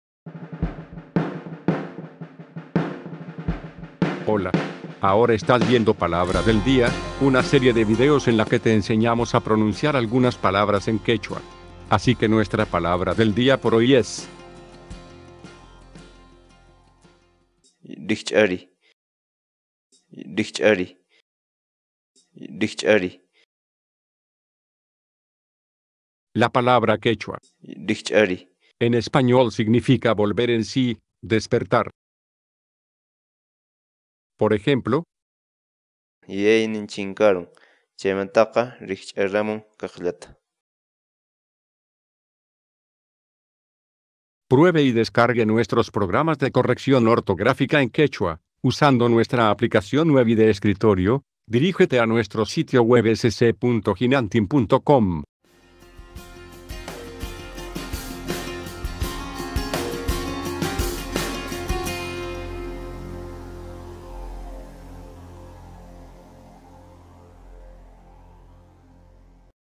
Como pronunciar rikch'ariy ‘volver en sí, despertar’
Social

Grupo de Investigación Hinantin: La entrada ‘rikch'ariy’ es parte de la base para el desarrollo de un diccionario hablado disponible gratuitamente para el idioma quechua.